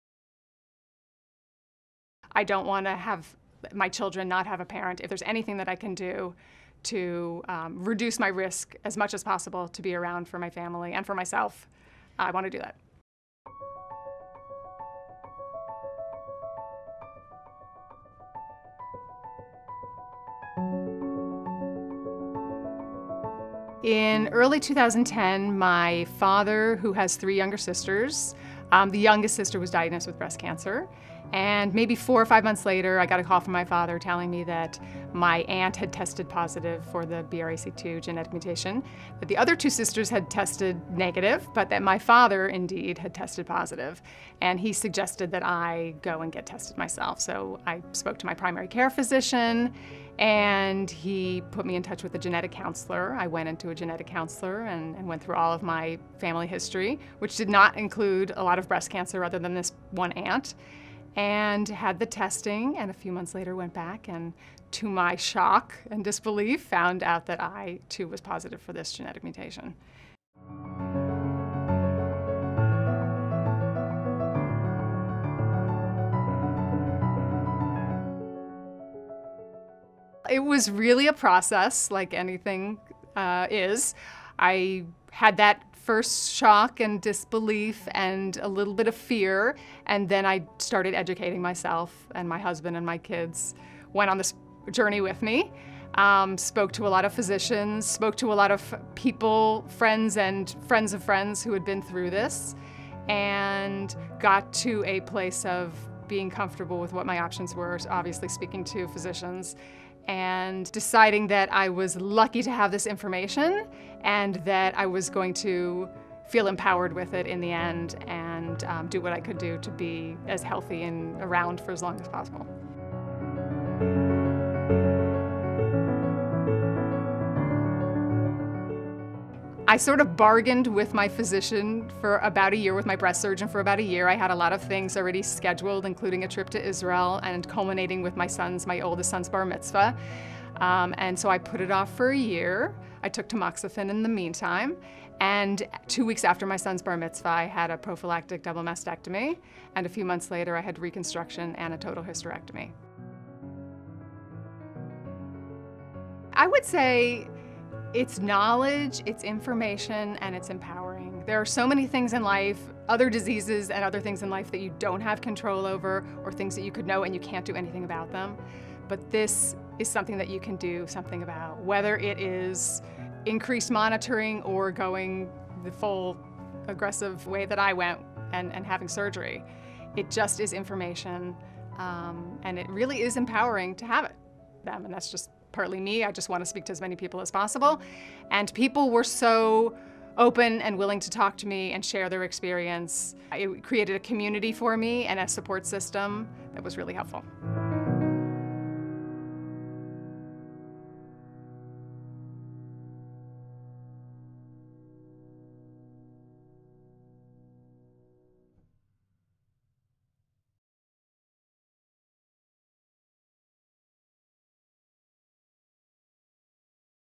Genetic Testing for the BRCA Gene | Being Jewish and Breast Cancer Risk
Genetic testing for the BRCA gene has led Melissa to aggressively take action to prevent her of developing breast cancer. Her story begins when she discovered that members of her family had tested positive for the BRCA2 gene through genetic testing.

Being Jewish can put you at high risk for breast cancer.  Jewish women and men of Eastern European descent (Ashkenazi Jews) often have a higher percent of the protein BRCA1 and BRCA, which went damaged, does not perform their proper function.